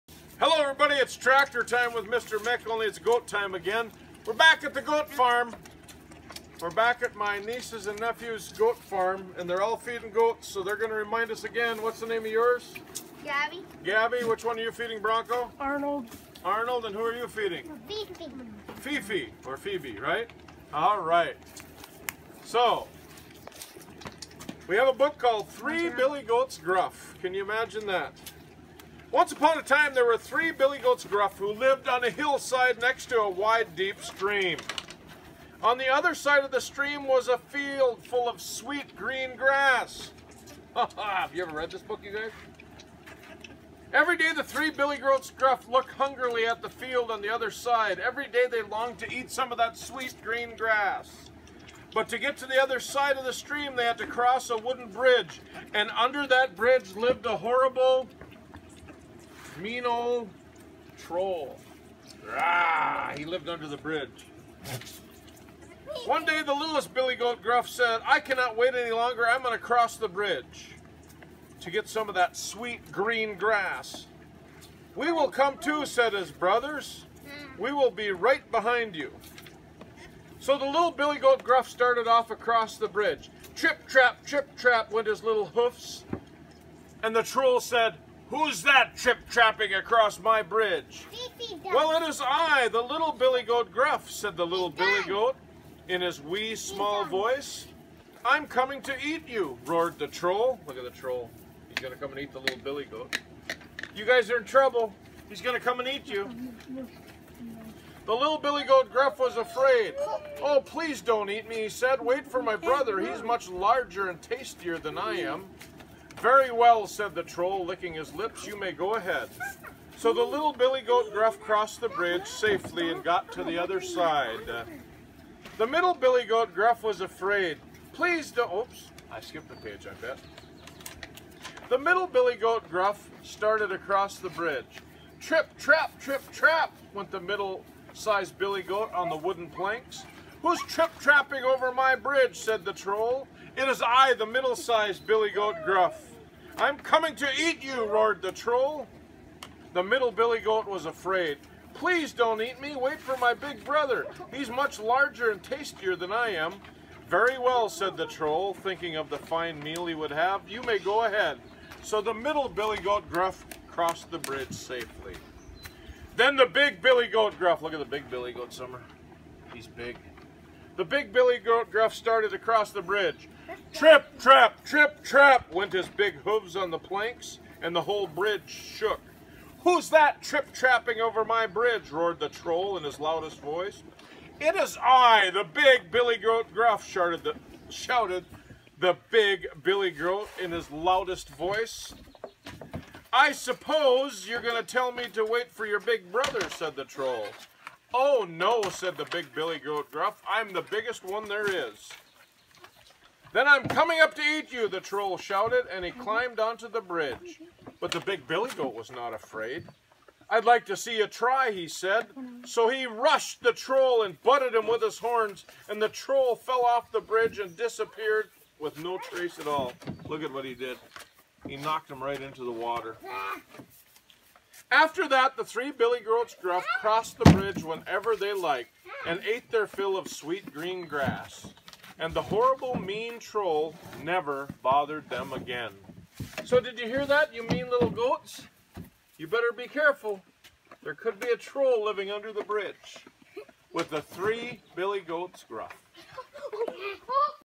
Three Billy Goats Gruff
Reading Three Billy Goats Gruff with Bronco, Clark, and Summer